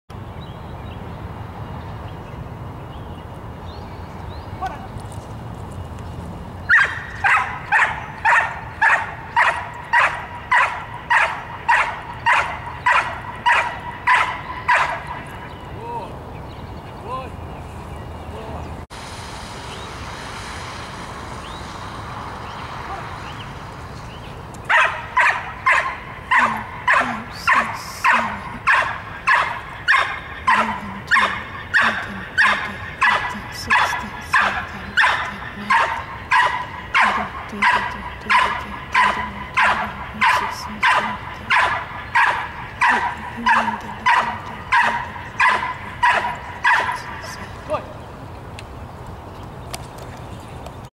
Rosa's K-9 Training (Champ 22 Months Old Hold & Bark)
Champ at 22 months old working on the hold & bark.